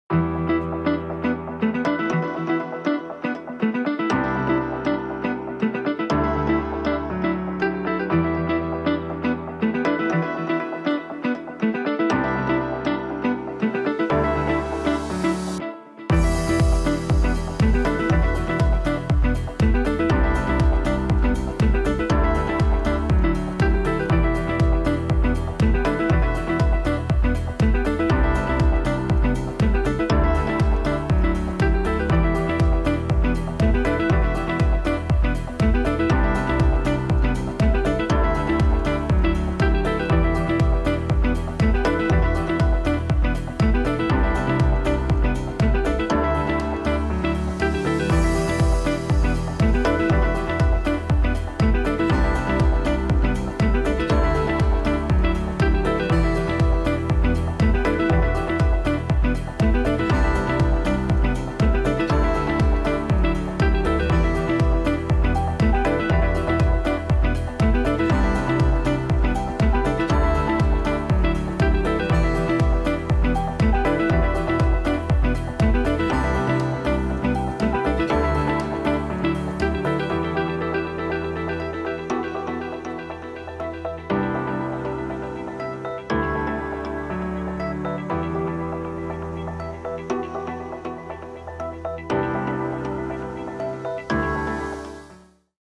17" Patented Detachable-Wheeled Laptop Briefcase, Leather, Black - THE CLINTON | McKlein USA
Traveling, especially for professionals, requires you to carry a lot with you. Luckily, the Clinton has all the storage space you need. With multiple organizers, padded laptop compartment, and our patented 2-in-1 detachable wheel and handle system, the Clinton will make sure you're prepared for whatever life throws at you and then some.